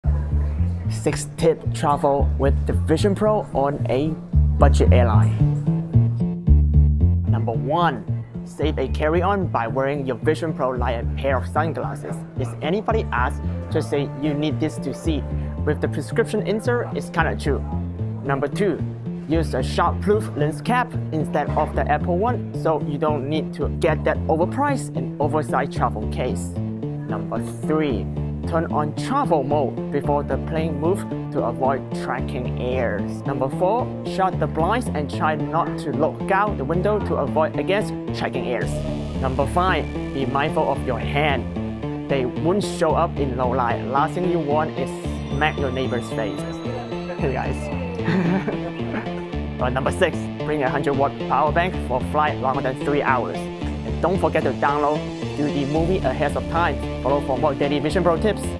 6 Travel Tips with 🍎 Vision Pro on a Budget ✈️
Most people think Vision Pro owners must be traveling in style on first-class or driving a Tesla Cybertruck with an expensive robot dog, thanks to TikTok …
Well, the reality is that most owners are so broken after $5000 ish spent, and they have to fly Spirit / budget airline instead 🥲 and live in the virtual world with instant ramen for survival …

Here are 6 EXTRA tips on traveling with Vision Pro that will help you save more money!

Let’s make it a norm, should we?